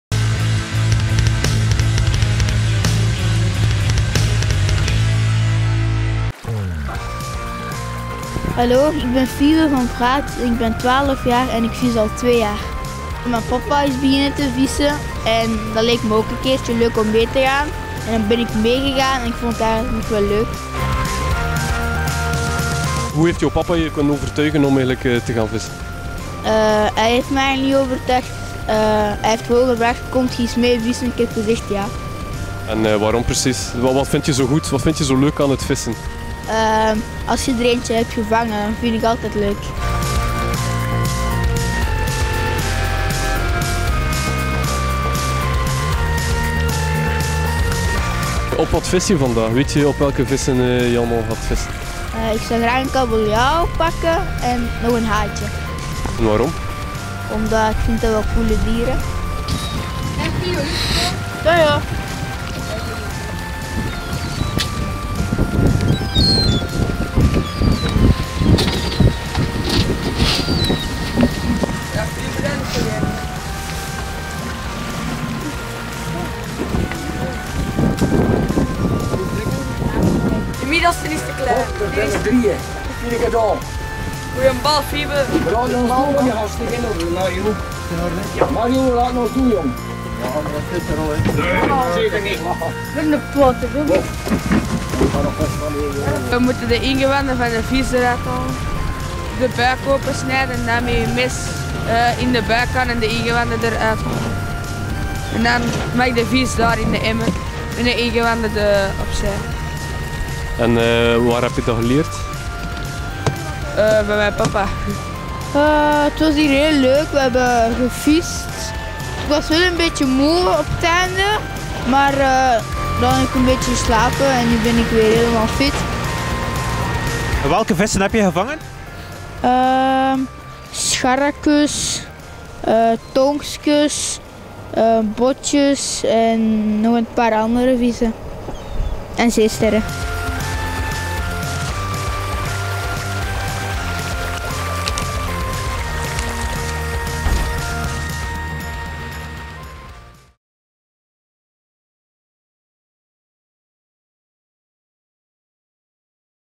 Extreme hobby's: vissen op zee
In onze reeks extreme hobby's gaan onze reporters op zoek naar de uitzonderlijke vrijetijdsbesteding van jongeren.

in deze aflevering begeven onze reporters zich samen met Fiebe de zee op om de golven te bedwingen en een grote vis aan de haak te slaan.